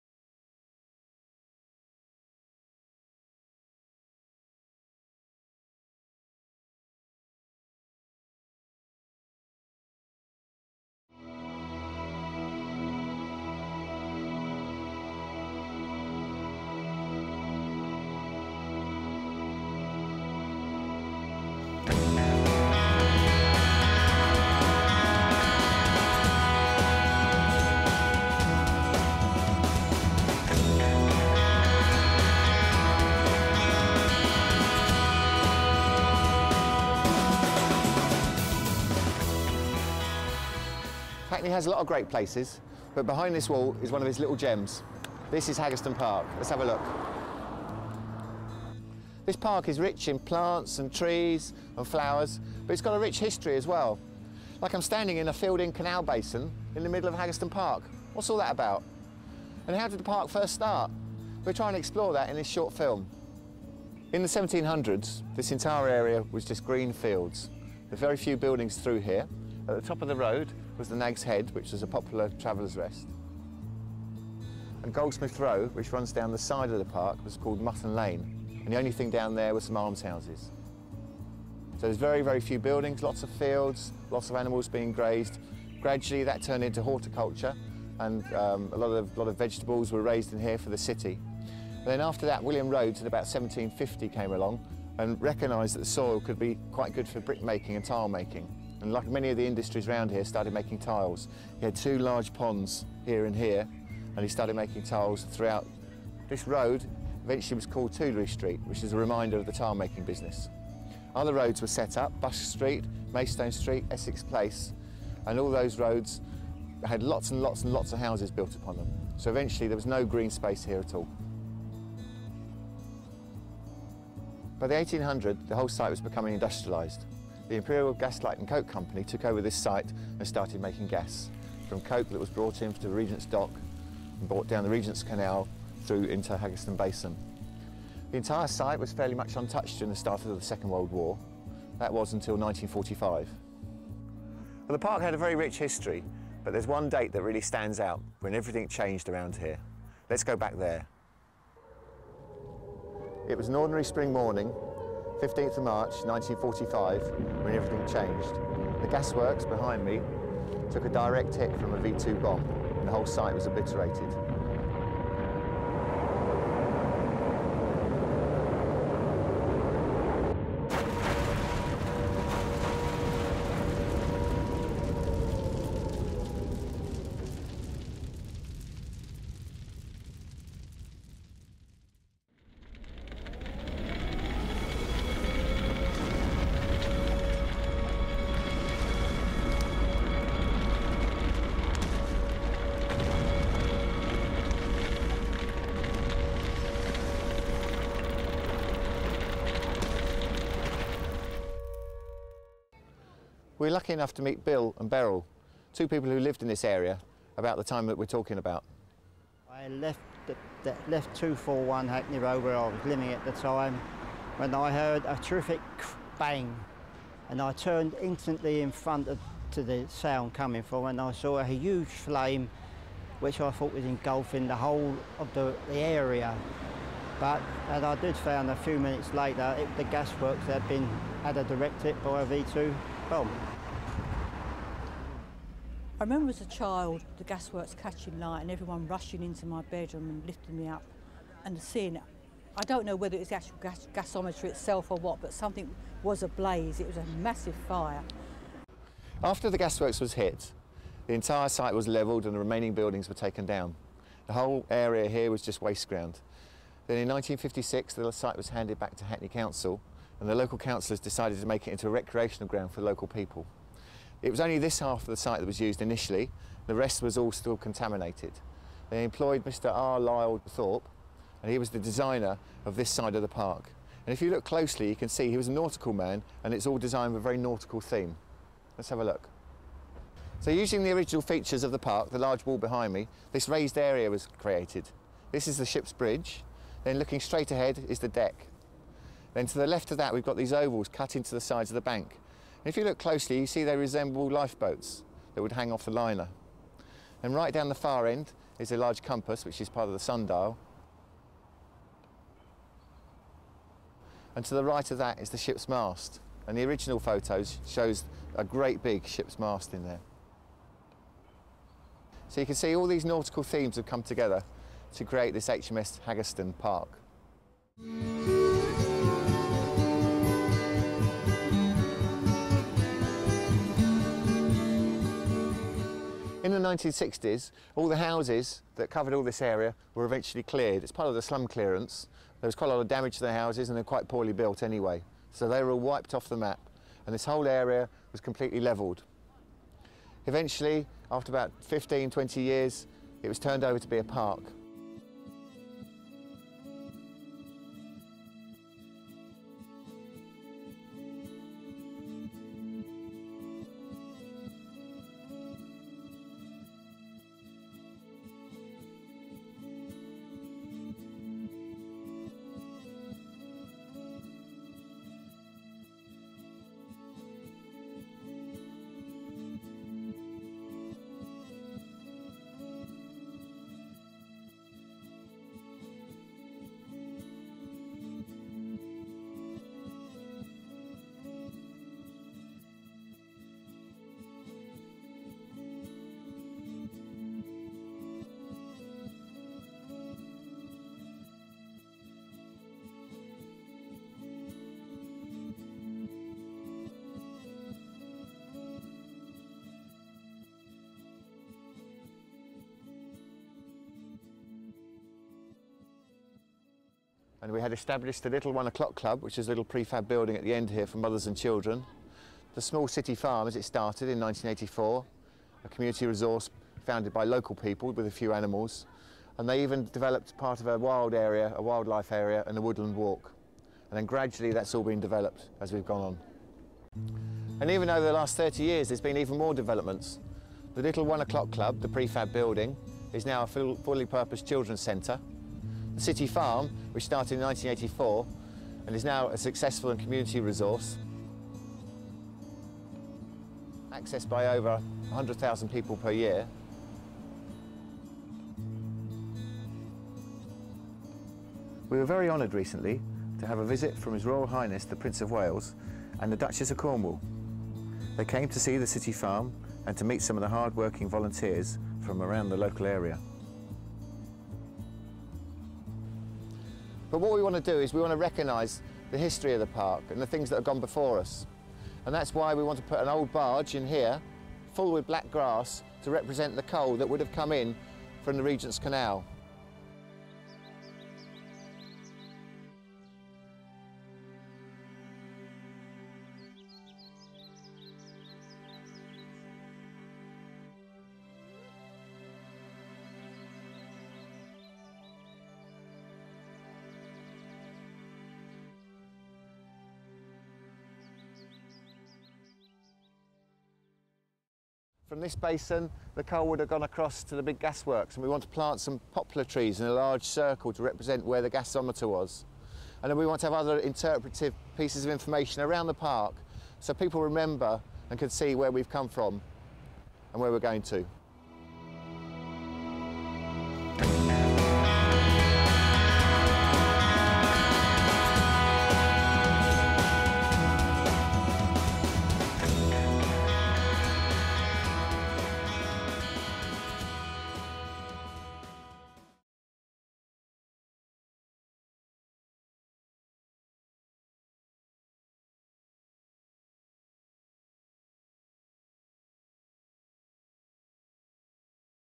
Haggerston Park History - a short film
A short film about the  history of Haggerston Park in East London made by two Hackney Council Gardeners.